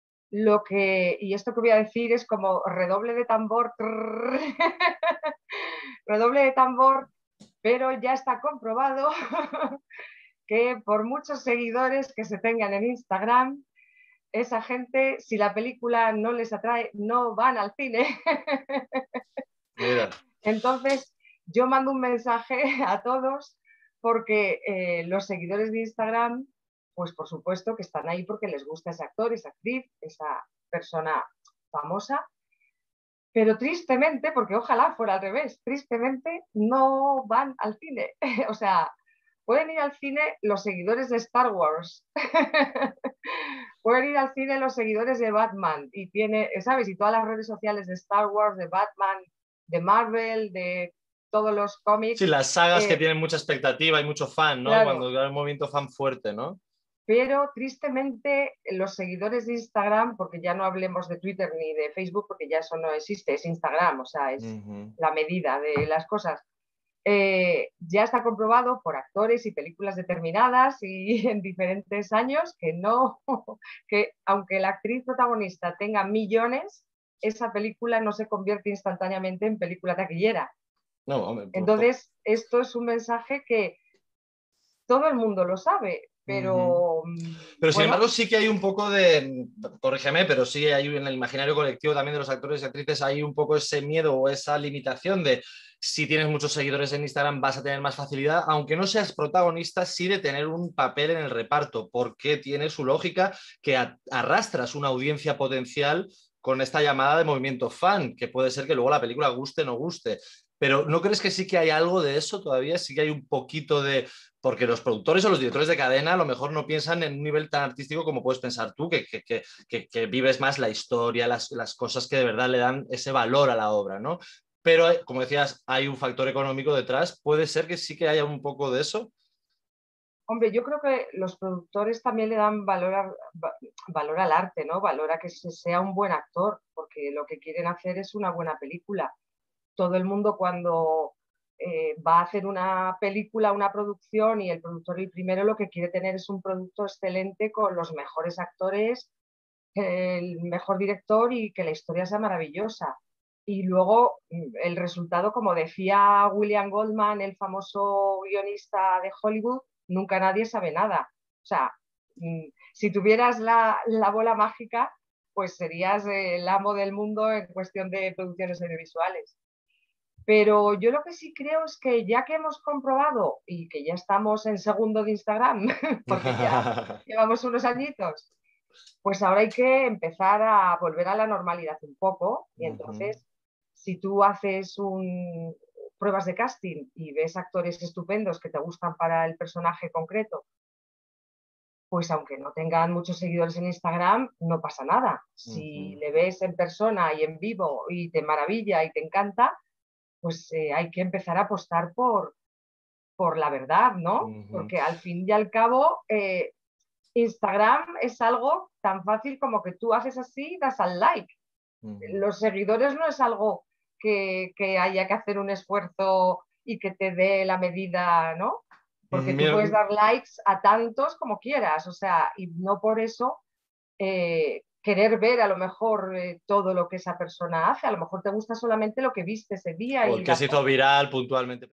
Seguidores de actores en Instagram.., ¿son determinantes en el éxito de un film? [Cristina Marinero]
Cristina Marinero es especialista en PR y comunicación de cine y producciones audiovisuales. Comenzó en 1997 con Beatriz de la Gándara y Fernando Colomo, con quienes ha trabajado en, entre otras, ‘Los años bárbaros’, ‘Atómica’, ‘A mi madre le gustan las mujeres’, ‘Al Sur de Granada’ o ‘Isla bonita’. Realiza campañas de comunicación para los Premios Goya, con Apache Films (‘Verónica’, ‘Quién te cantará’, ‘La llamada’, ‘Sesión salvaje’, ‘Los europeos’…), y para los documentales ‘Anatomía de un dandy’ y ‘Desenterrando Sad Hill’, consiguiendo ambas la nominación.

Con la Academia de Cine ha sido Responsable de Protocolo y Comunicación para los nominados, en las ediciones de los Premios Goya de 2010 (la más exitosa, de audiencia y crítica), 2011 y 2015. Ha trabajado en Cartel y Drive Entertainment, con producciones como ‘Solo quiero caminar’, ‘Café solo o con ellas’ y ‘Cuéntame’ (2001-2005) o el musical ‘Hoy no me puedo levantar’.

Cristina es miembro de la Academia de las Artes y Ciencias Cinematográficas de España y AISGE.